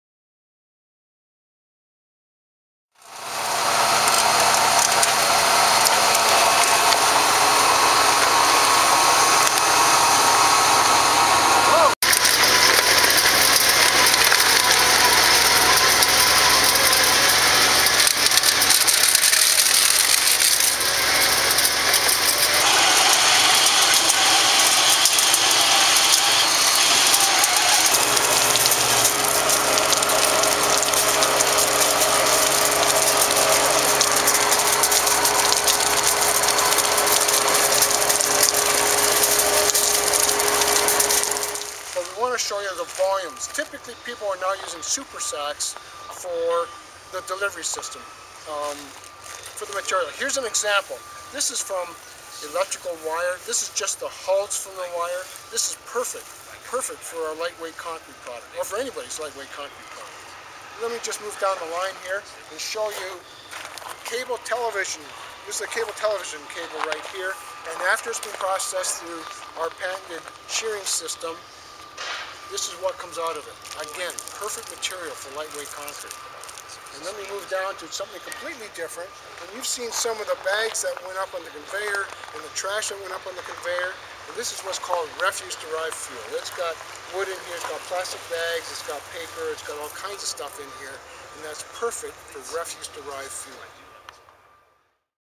Non Recyclable Waste Processing
What to do with the solid waste that can't be recycled?  Create repurposed product. Inventive Environmental Solutions does that by processing the solid waste  through its patent pending shearer/shredder systems. The resulting repurposed product can be used for Refuse Derived Fuel, lightweight concrete and other uses.